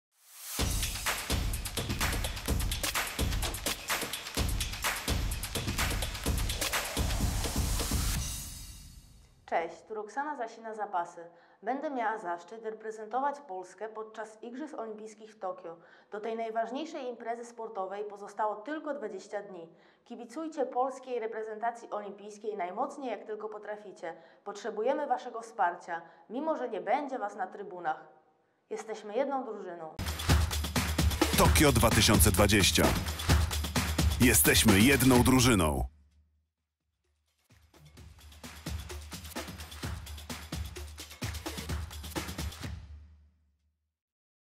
20 dni do Tokio 2020 – Roksana Zasina
Do Igrzysk XXXII Olimpiady Tokio 2020 pozostało tylko 20 dni. Kibicujcie Polskiej Reprezentacji Olimpijskiej – zachęca Roksana Zasina, zapasy.